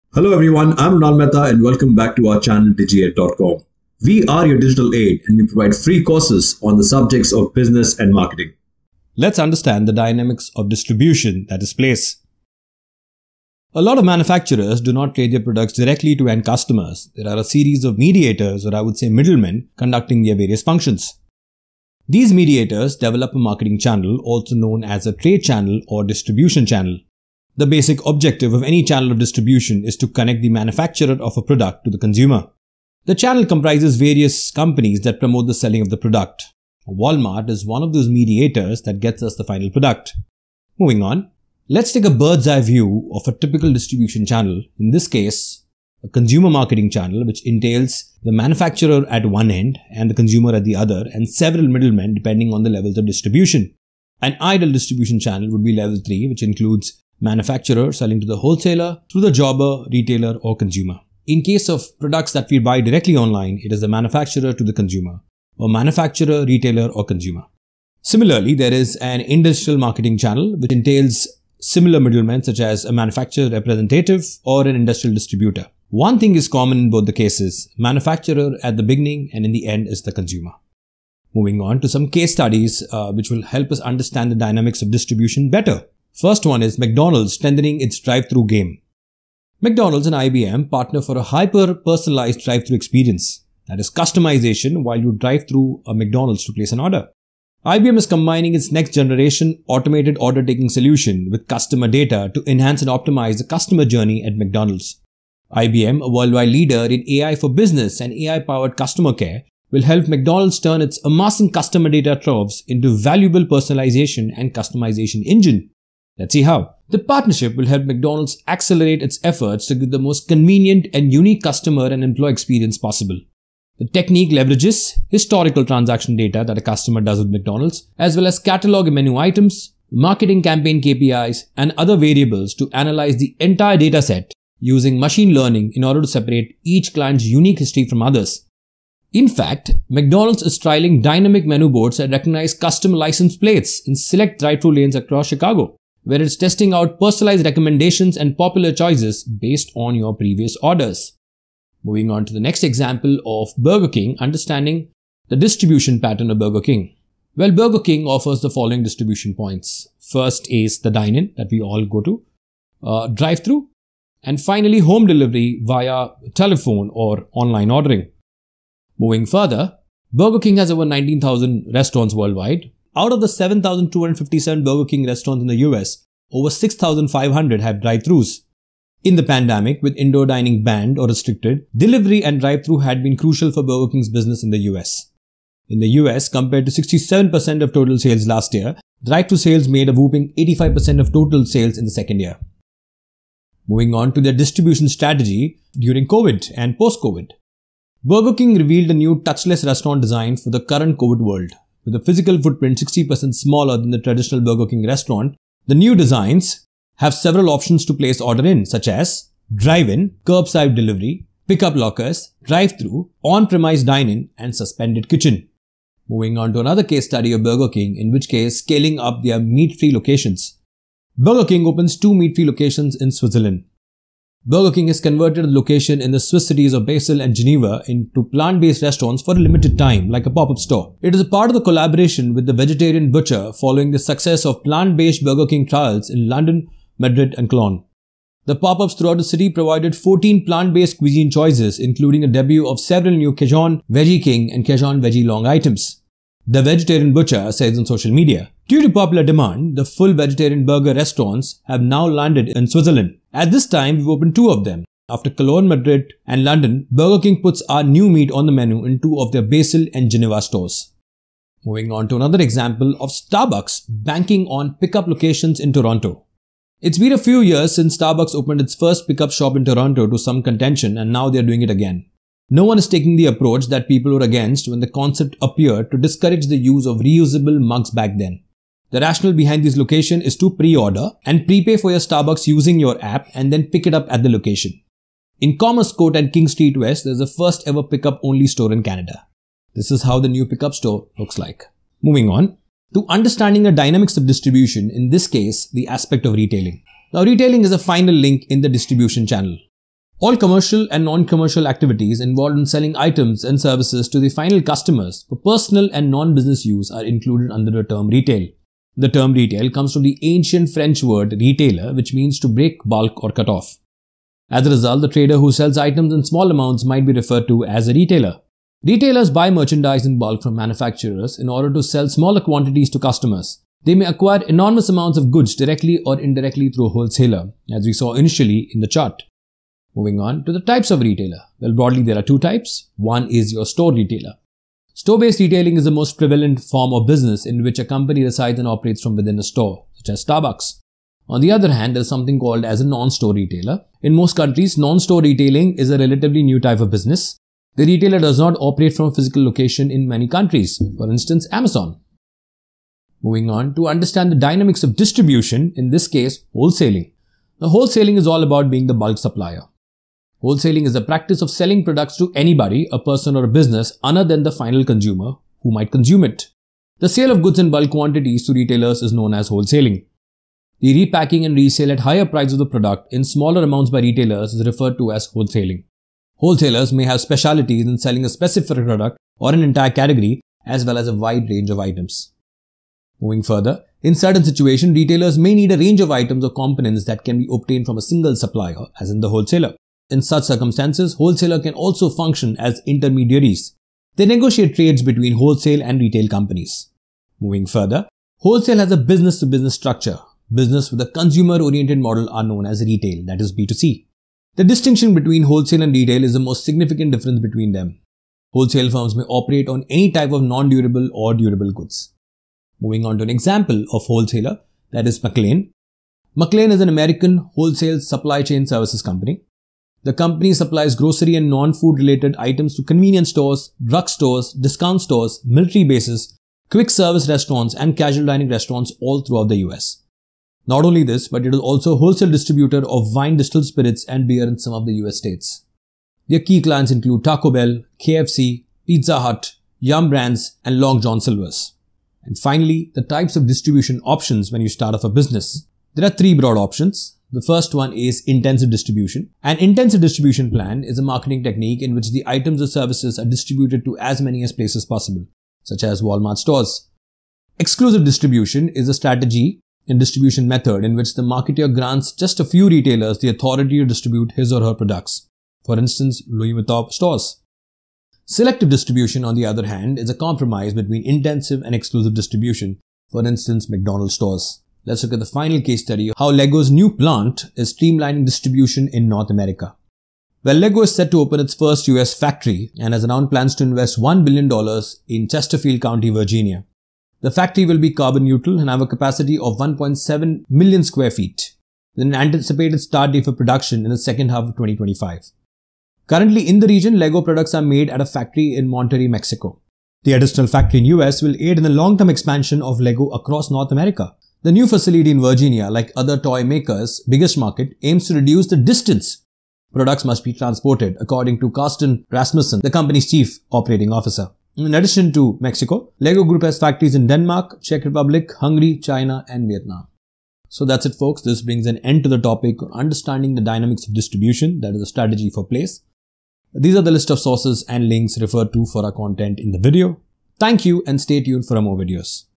The Dynamics of Distribution and how to distribute in an ever changing world
Understand the Dynamics of Distribution (Place)
A lot of manufacturers do not trade their products directly to the end-users; there are a series of mediators conducting their various functions.
These mediators develop a marketing channel, also known as trade channel or distribution channel. The basic objective of any channel of distribution is to connect the manufacturer of a product to the consumer.
The channel comprises various companies that promote the selling of the product.

 Case Studies
McDonald's – Strengthening its Drive-Thru Game
McDonald's And IBM Partner For Hyper-Personalized Drive-Thru Experience
IBM is combining its next-generation Automated Order Taking (AOT) solution with customer data to enhance and optimize the consumer journey at McDonald's.
The partnership will help McDonald's accelerate its efforts to give the most convenient and unique customer and employee experience possible.
The technique leverages historical transaction data, as well as catalog and menu items, marketing campaign KPIs, and other variables to analyze the entire dataset using machine learning in order to separate each client's unique history from others.
McDonald's is trialing dynamic menu boards that recognize customer license plates in select drive-thru lanes across Chicago, where it is testing out personalized recommendations and popular choices based on previous purchases.

Burger King products are available through the following distribution points
DINE IN
DRIVE THRU
HOME DELIVERY

There exists over 19,000 Burger King restaurants throughout the world
Out of the 7,257 Burger King restaurants in the U.S., over 6,500 have drive-thrus.
In the pandemic, with indoor dining banned or restricted, delivery and drive-thru had been crucial for Burger King’s business in the U.S.
In the U.S., compared to 67% of total sales last year, drive-thru sales made a whopping 85% of total sales in the second year
Burger King – Distribution (Place Strategy Post COVID)
Burger King revealed a new touch less restaurant designs for the current covid world. With a physical
 footprint 60% smaller that the traditional Burger King restaurant building, new designs will
It has several options to place an order; Drive-In, Curb-side Delivery, Pick up lockers, Drive-thru,
 On-premise Dine-in and Suspended Kitchen

Burger King – Scaling Up Meat-Free Locations
Burger King Opens Two Meat-Free Locations in Switzerland
Burger King has converted its locations in the Swiss cities of Basel and Geneva into plant-based restaurants for a limited time. It is part of a collaboration with The Vegetarian Butcher following the success of the plant-based Burger King trials in London, Madrid, and Cologne.
The pop-ups throughout the city provided 14 plant-based cuisine choices, including the debut of several new Cajun Veggie King and Cajun Veggie Long items.
And this time, we even opened TWO of them! After Cologne, Madrid and London, Burger King puts our ‘new meat’ on the menu in two of their Basel and Geneva stores."

Starbucks – Banking on “Pick-Up” locations in Toronto
Starbucks is opening its second pick-up location in Toronto
It's been a few years since Starbucks opened its first pick-up shop in Toronto to some contention, and now they're doing it again.
No one is taking the approach that people were against when the concept appeared to discourage the use of reusable mugs back then.
The rationale behind these locations is to pre-order and pre-pay for your Starbucks using their app, then pick it up at the location.
In Commerce Court at King Street West, there's a first ever pick-up only store in Canada.

Understand the Dynamics of Distribution – Retailing
Retailing – The Final Link in the Distribution Channel
All commercial and non-commercial activities involved in selling items and services to the ultimate (final) customers for personal and non-business use are included under the term "retail."
The term "retail" comes from the ancient French word "retailier," which means to break bulk or cut off.
As a result, a trader who sells items in small amounts might be referred to as a retailer.

This video is on Understand the Dynamics of Distribution (Place) and it Objectives and it has the following sub-topics.

Time Stamps

0:00 Introduction to Distribution
0:16 Understand the Dynamics of Distribution (Place)
1:28 Understand the Dynamics of Distribution – Case Studies
4:59 Understand the Dynamics of Distribution – Retailing
5:36 Types of Retailers
6:00 Understand the Dynamics of Distribution – Wholesaling
7:29 Types of Distribution Options